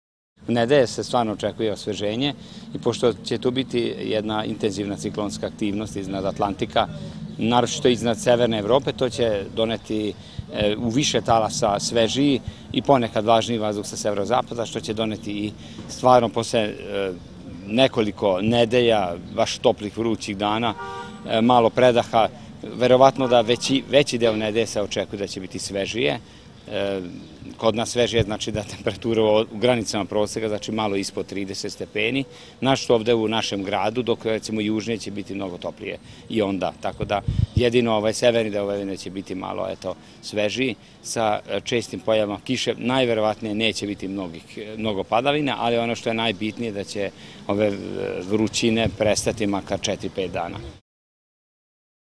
Od nedelje zahlađenje
U poslednjih 40 godina ovako visoke temperature nisu zabeležene za protekli period, rekao je za yueko saradnik Republičkog hidrometeorološkog zavoda, Geza Kovač.

Sutra nas čeka topao dan, a nakon toga sledi blago zahlađenje sa temperaturom ispod ili oko 30 stepeni celzijusa.